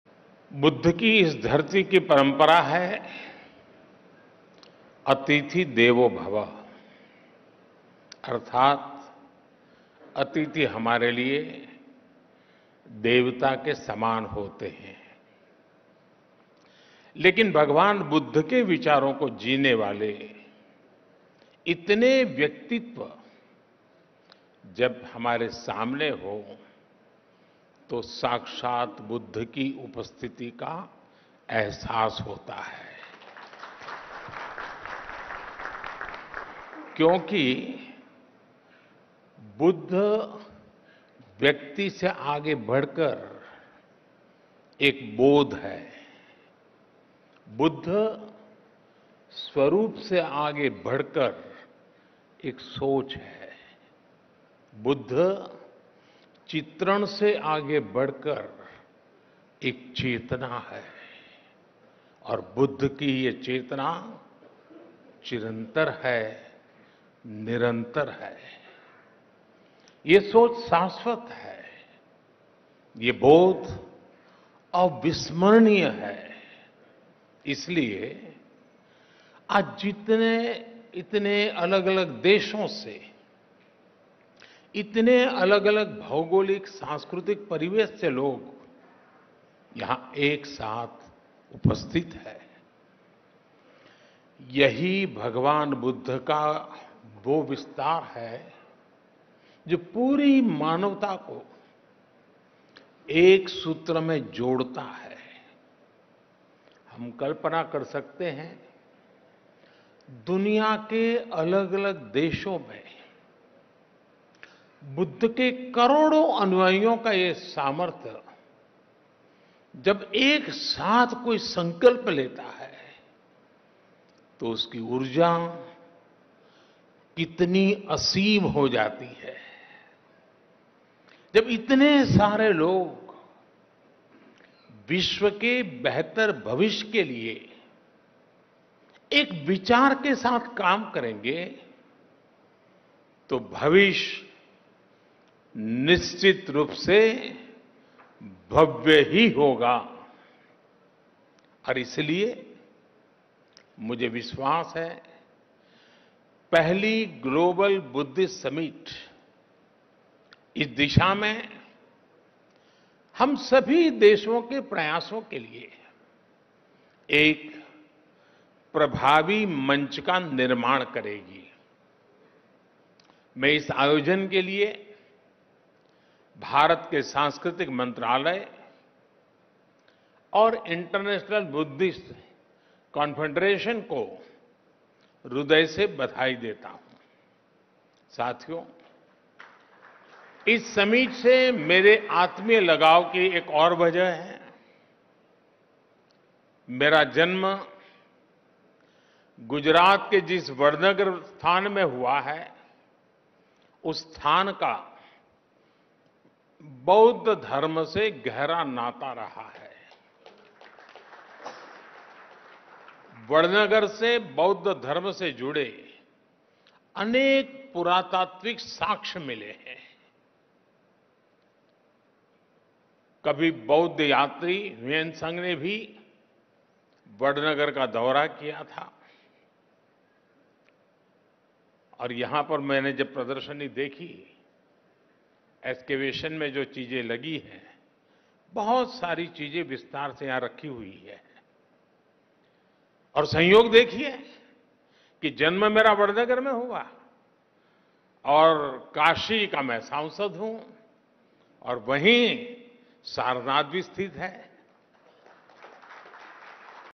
PM Modi ने बताया - Global Buddhist Summit से उन्हें क्यों है आत्मीय लगाव?
PM addressed the Global Buddhist Summit where he offered flowers to the statue of Lord Buddha and as a tribute he offers Chivar Dana to 19 monks. He reiterated the Buddhist principle of "Buddha is beyond individual, It is a perception.